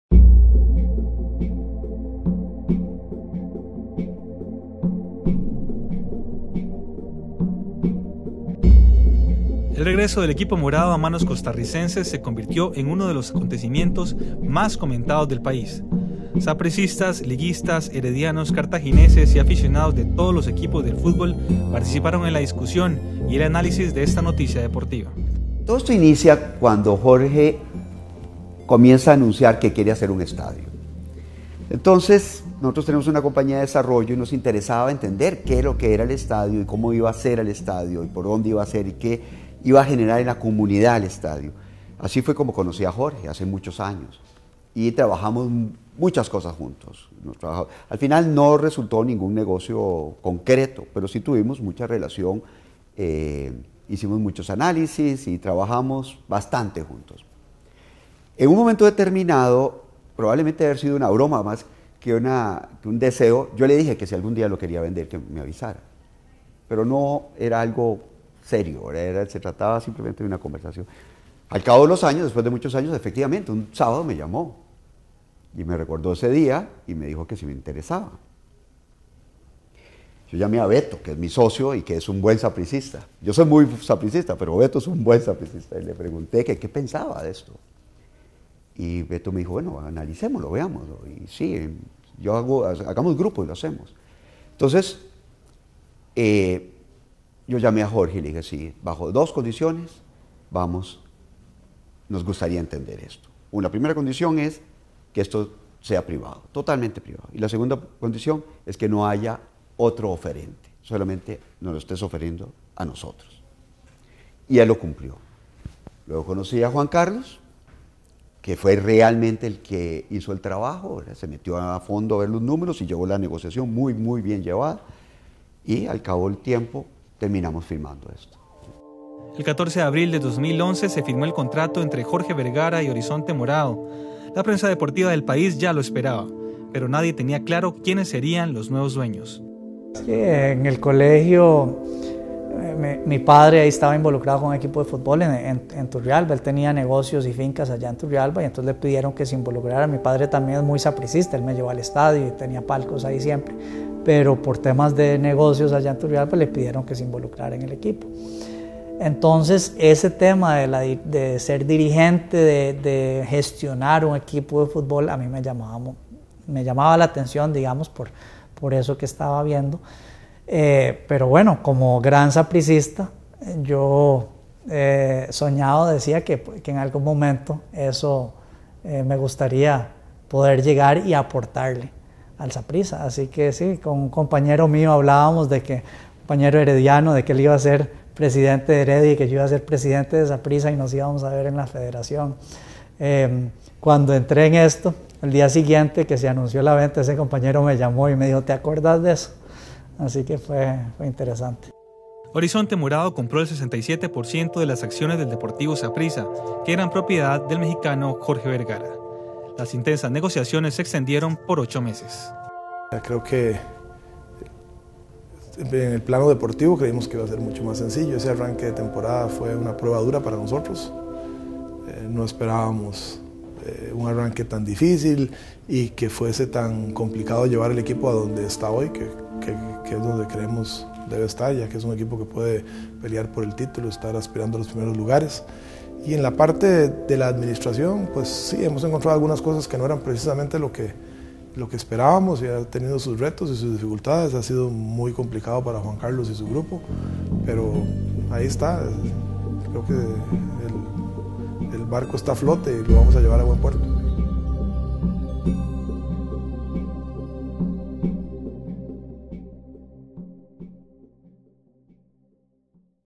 ¿Quiénes son los dueños del Deportivo Saprissa?
Juan Carlos Rojas, Alberto Raven y Édgar Zurcher, los dueños del Deportivo Saprissa, seleccionados como uno de los personajes del año 2011 por la revista Dominical de La Nación.